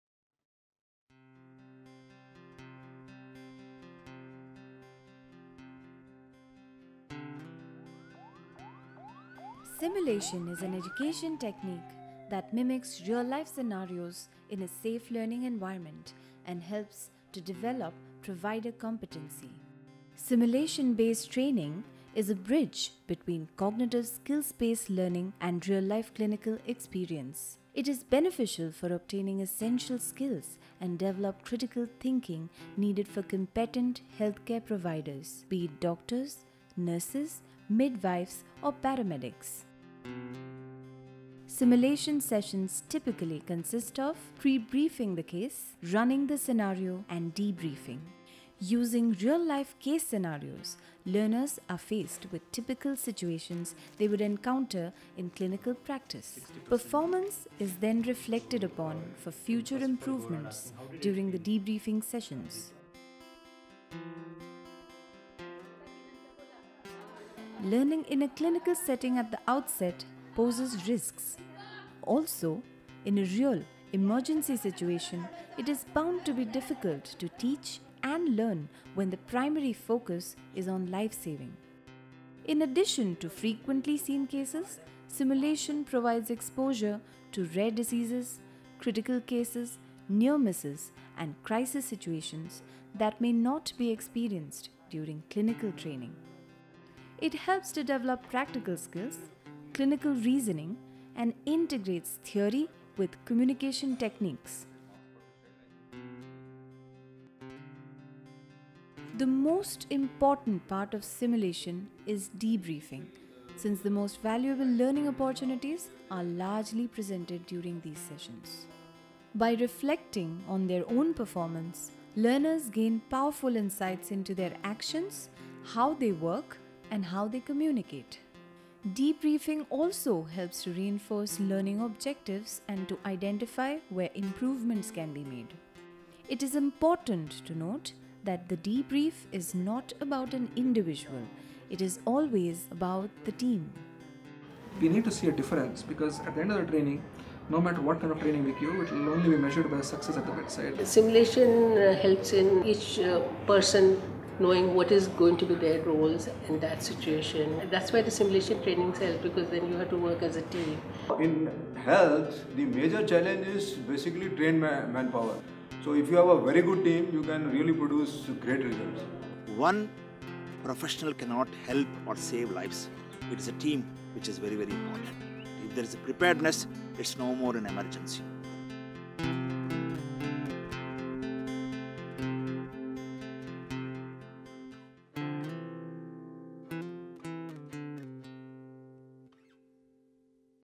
The opportunities of simulation based education
A short film detailing out the opportunities of simulation based education. Also included is an overview of the 'Circle of learning' and  steps of simulation based education.